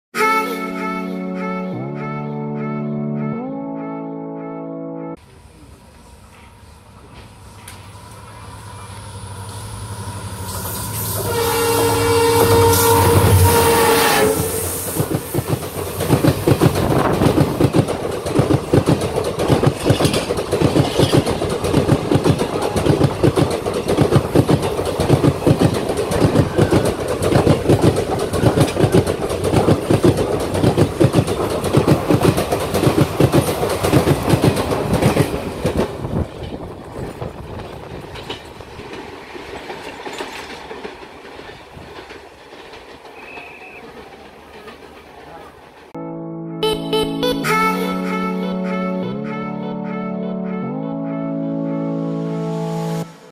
19259 / Kochuveli-Bhavnagar Express at Nivsar | Konkan Railways
The 19259 / 60 Kochuveli-Bhavnagar Express is anExpress train belonging to Indian Railways - Western Railway zone that runs between Kochuveli and Bhavnagar Terminus in India.

It operates as train number 12959 from Kochuveli to Bhavnagar Terminus and as train number 12960 in the reverse direction serving the states of Kerala, Karnataka, Goa, Maharashtra and Gujarat

The 19259 / 60 Kochuveli-Bhavnagar Express has a total of 19 coaches which includes one AC 2 tier, three AC 3 tier, seven Sleeper class, six General Unreserved & two SLR (Seating cum Luggage Rake) Coaches. It does not carry a Pantry car coach.[3]
As is customary with most train services in India, Coach Composition may be amended at the discretion of Indian Railways depending on demand